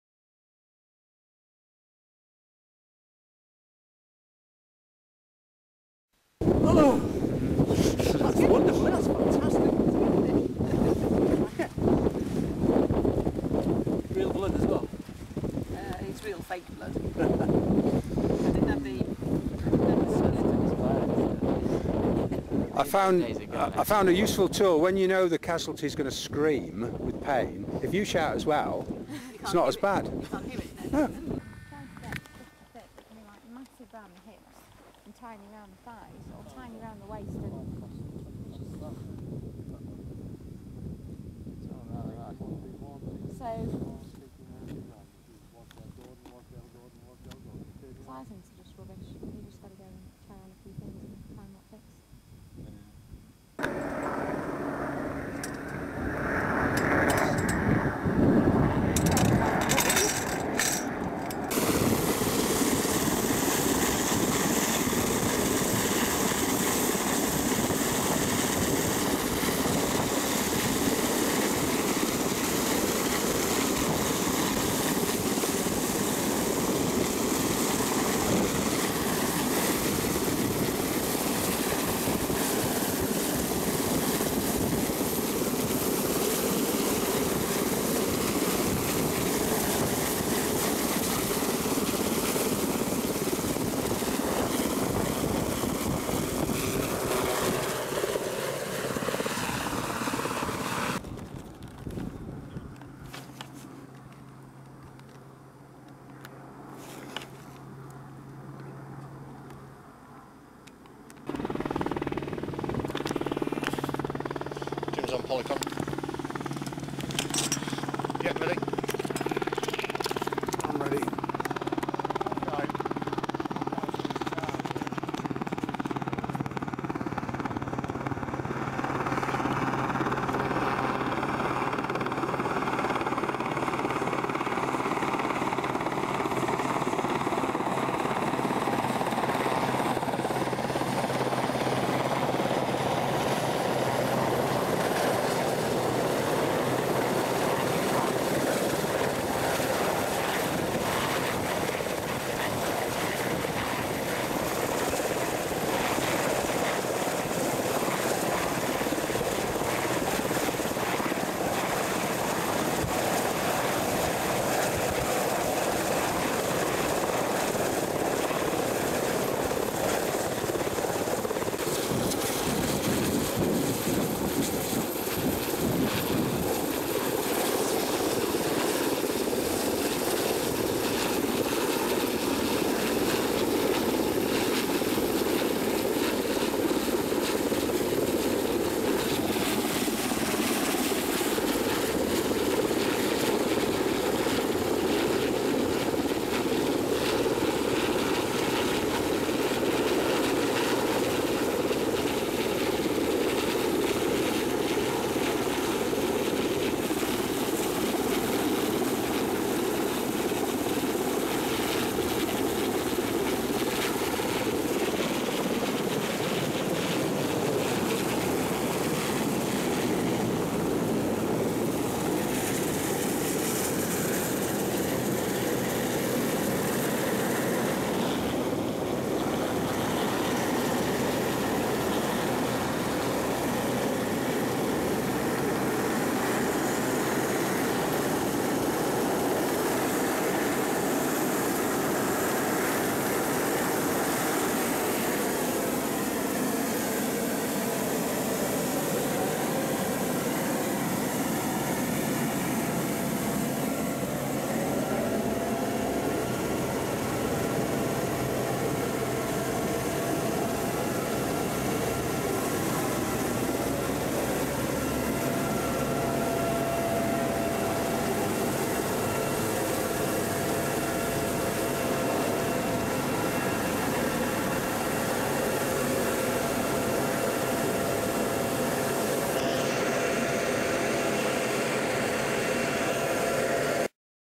Wasdale Mountain Rescue Team - Heli Training 2006
During a full day practice on search and casualty care, RAF Boulmer came across to carry out some traing - ther is some footage of what it is like to be a casualty starpped into a stretcher and winched into the aircraft